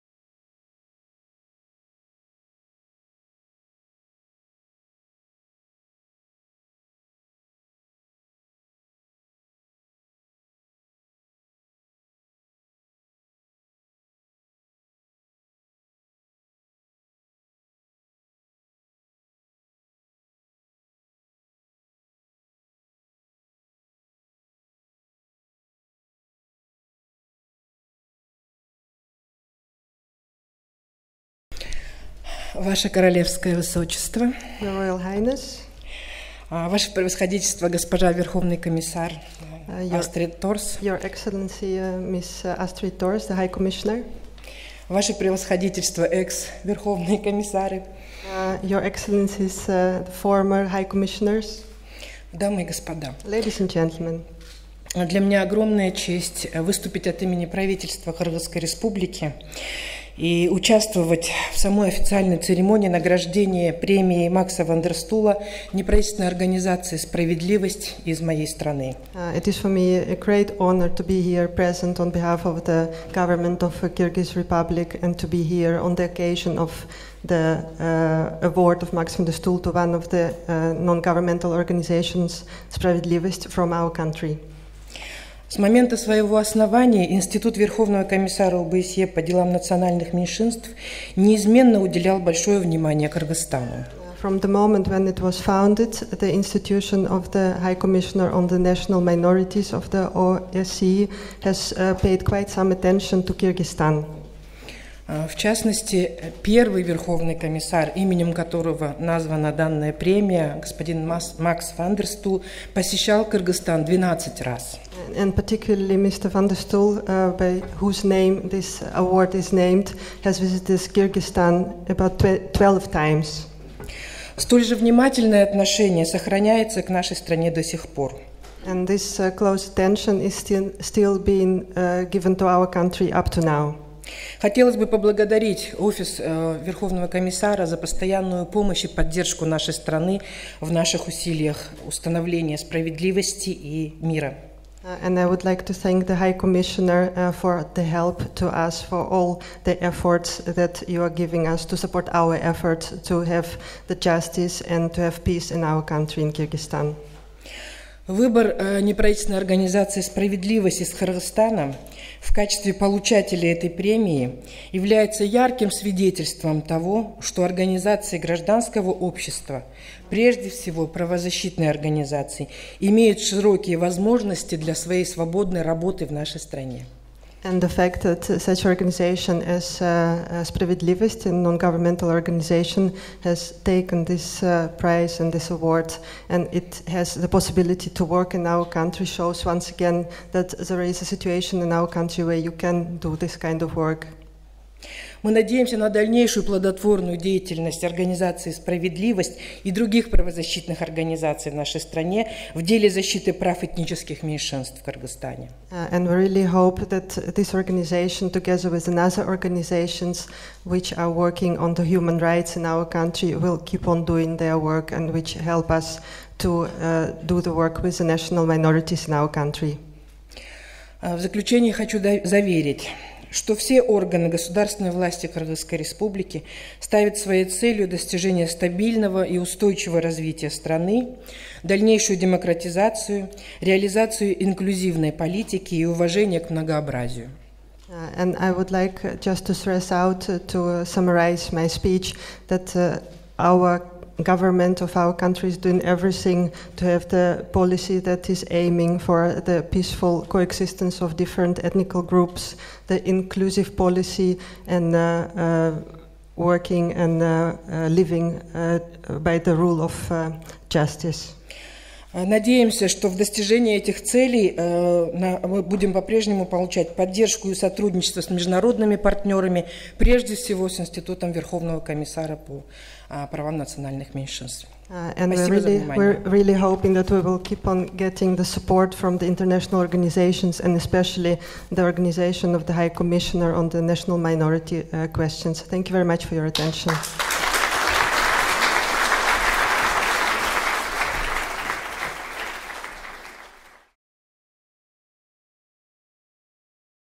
Speech by Mira Karybaeva, Presidential Administration of Kyrgyzstan
Speech by Mira Karybaeva, Head, Department of ethnic, religious policies and interaction with civil society, Presidential Administration of Kyrgyzstan, at the Max van der Stoel Award 2014.

The Max van der Stoel Award 2014 was presented to Spravedlivost, a human rights NGO from Kyrgyzstan, during a ceremony in The Hague, 2 October 2014. The Max van der Stoel Award is presented every two years to is presented every two years to a person, group or institution for extraordinary and outstanding achievements in improving the position of national minorities in the OSCE participating States.